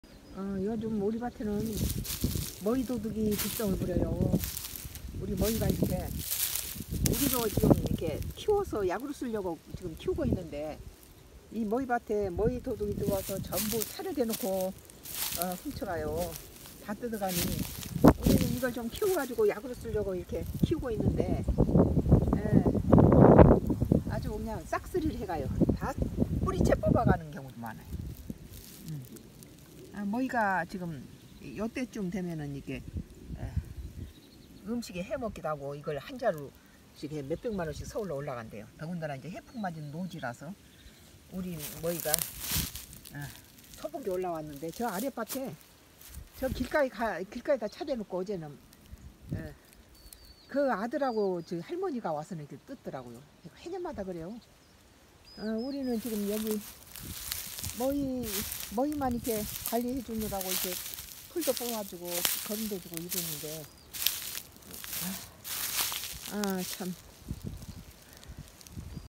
우리 머위 밭에~~~머위도둑!@sungodcross
우리 머위 밭에~~~머위도둑! 10년 귀촌 중에~~~

요근래 2~3년 사이에 ! 감자밭이나 고구마밭이 도둑을 맞아서~~감자는 종자값도 건지지 못하고 마트에서 감자를 사서 먹었다~!
그런데~~~우리가족의 1년 양식인 머위밭에 ! 해년마다~~자동차를 대동하고 도로가에 세워두고~~~우리 머위밭에 새로 나오는 머위순를 모두 뜯어가고 있었다~~!

 아들이 운전하고 해년마다 우리 머위밭에 들어와서 머위 뿌리까지 캐어가던 할머니가~~!
올해도~~~어김없이 낮 12시 반에 우리가족이 점심식사하러 가는 시간에 머위도둑은~~~우리머위 밭을 털러왔다~~!

 바닷가 해풍맞은 노지 머위가 ~~서울서는 최고값으로 한자루에 몇백만원씩 간다니`~~~! 요즘 갑자기 마을에 원수들 하수인들이 우리뒷집으로 입점한 뒤~~~우리밭에 쓰레기를 던지고 농작물이~~~ 의도적으로 훼손당하고 마을이 쓰레기 천지가 되고있으며 독극물이 난무하게 되었고~~!

 조용한 어촌마을에~~~전쟁터에서나 살상용으로 터뜨리는 폭죽이 한밤 중이나 새벽 3시에 우리집지붕위로 터지는데~~!신고받은 경찰은 제일먼저 와서~~우리 100년된 농가주택에 CCTV 있는가?

확인하며~~~ 나보고 그들이 Y하수인 윗집에서~~ 불총을 우리집으로 쏘아대는 증거를 대라고 했다~~! 이런 악한자들때문에` ! 평화로운 어촌마을이 갈수록 농사짓기가 어려워진다~~~!@sungodcross